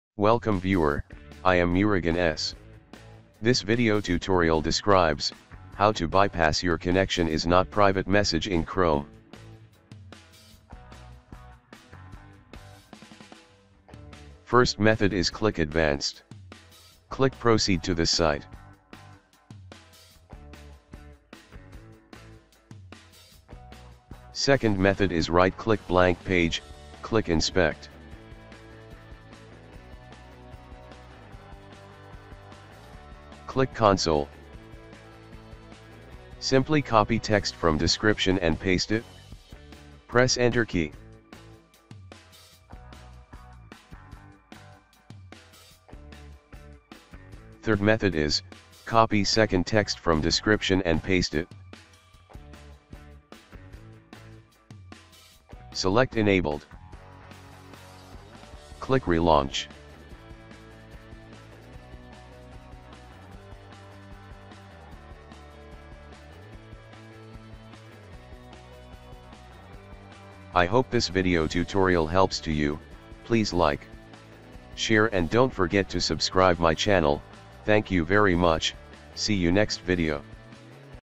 Chrome : How To Bypass “Your connection Is Not Private” Message
This Tutorial Helps to Chrome : How To Bypass “Your connection Is Not Private” Message

sendCommand(SecurityInterstitialCommandId.CMD_PROCEED)
chrome://flags/allow-insecure-localhost